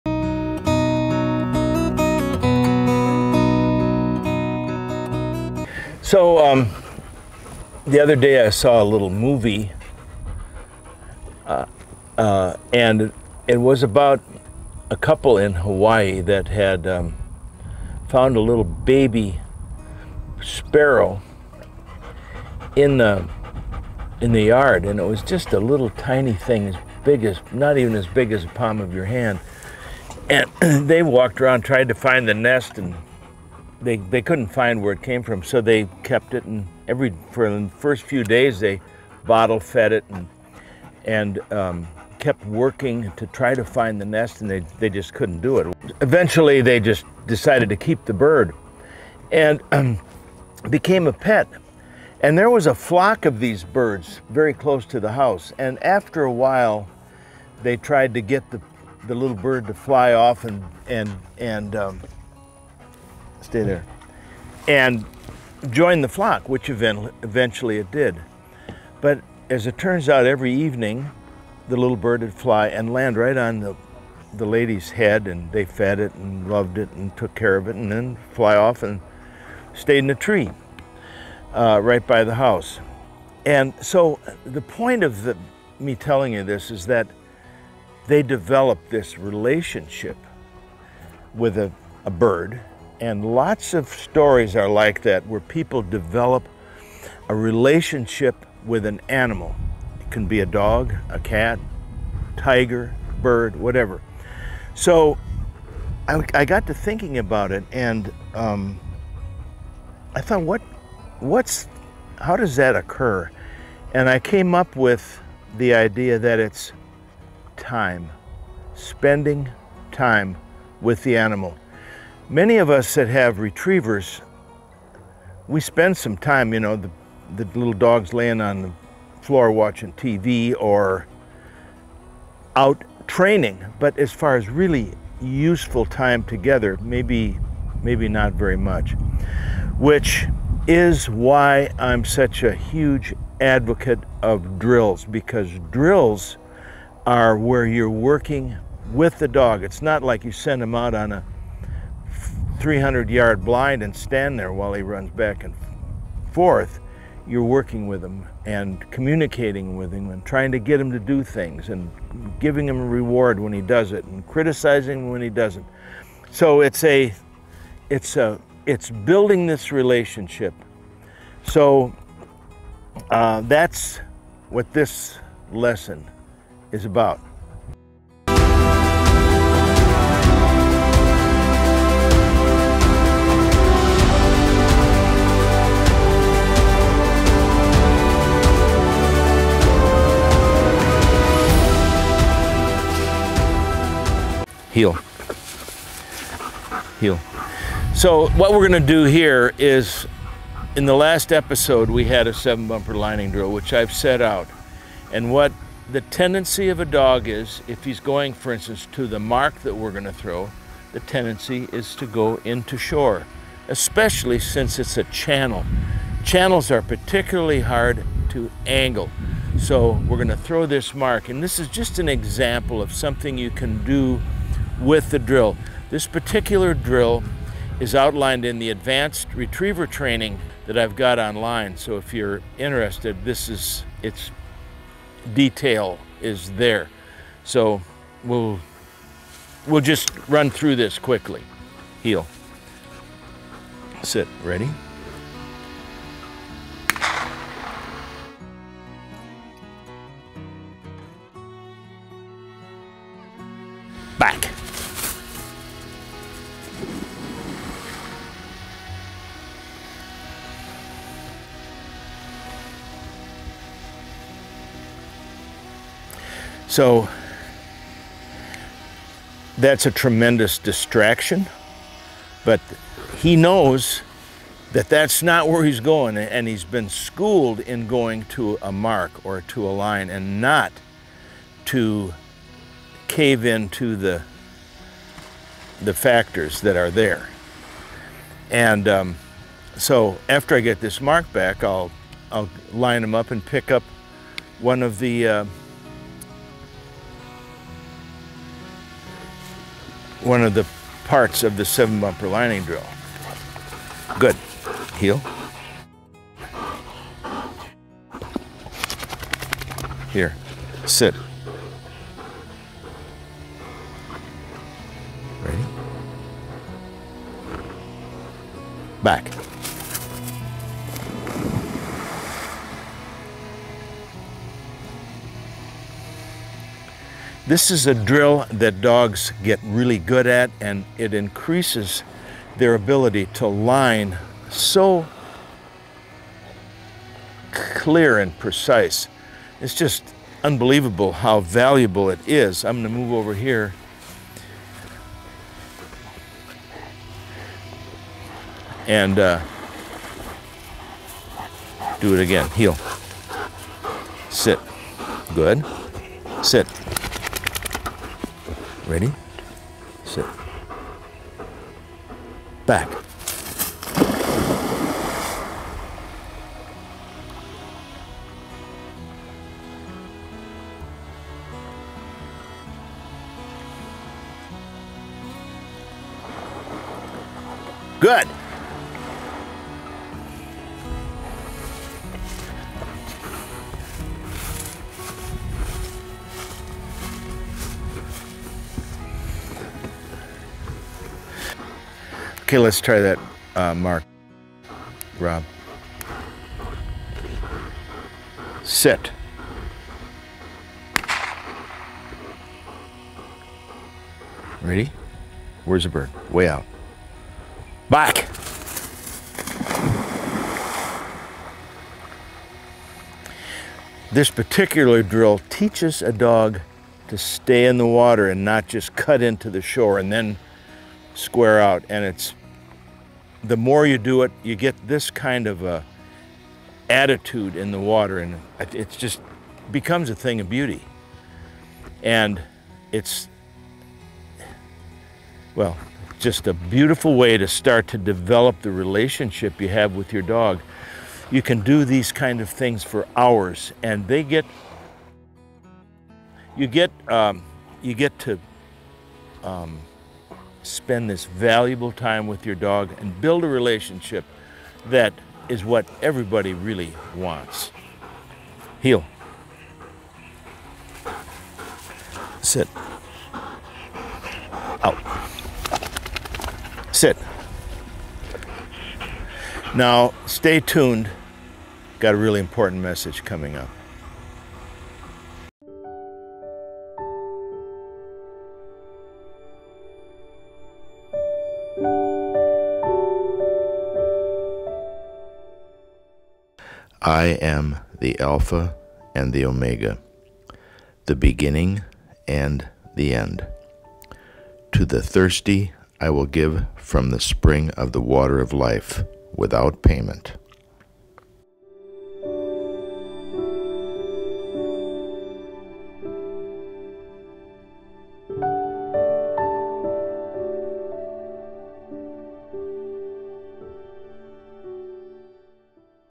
The Best Water Lining Drill with Bill Hillmann (2)
Teaching lining in the water for a retriever is shown with a drill Bill uses
regularly when advancing a dog in retriever training. Part 2 takes one step further.
SHOW LESS